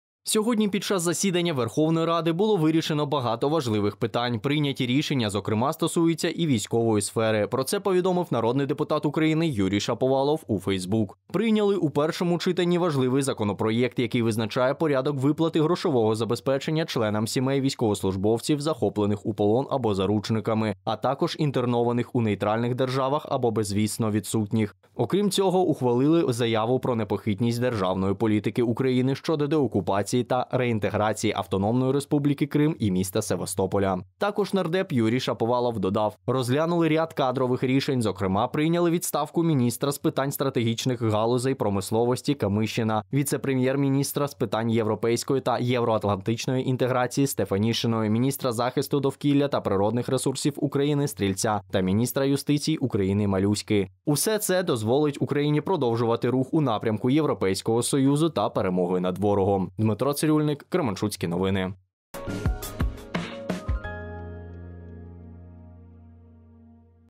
У Верховній раді України відбулося важливе голосування
У Верховній раді України відбулося важливе голосування, що стосувалося соціальної, військової та кадрової політики